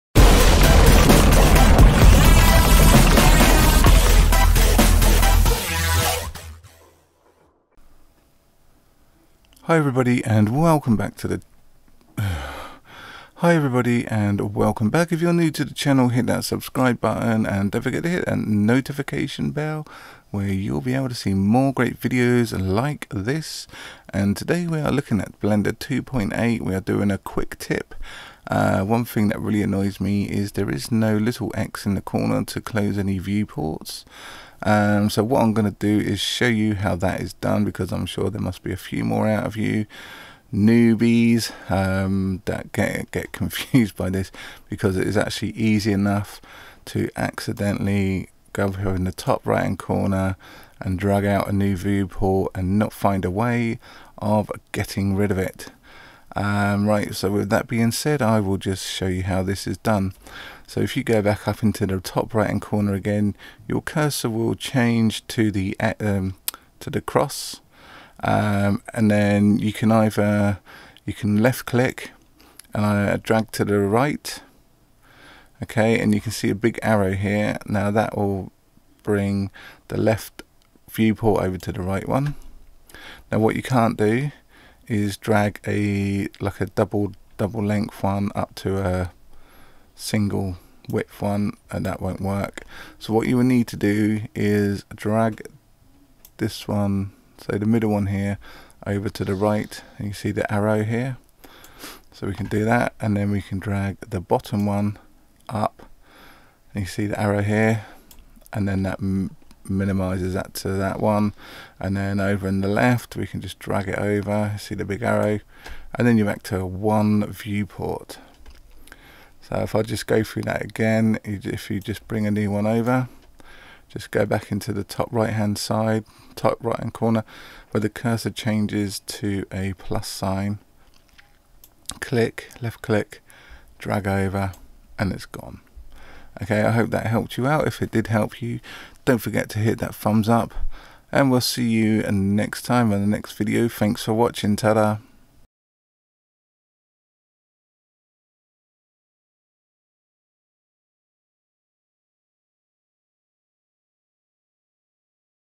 Blender 3D Quick tip: Closing Viewports
I'm sure many of you have problems closing viewports in Blender 3D, I know I used to. So I though I would share my knowledge and show you the secret!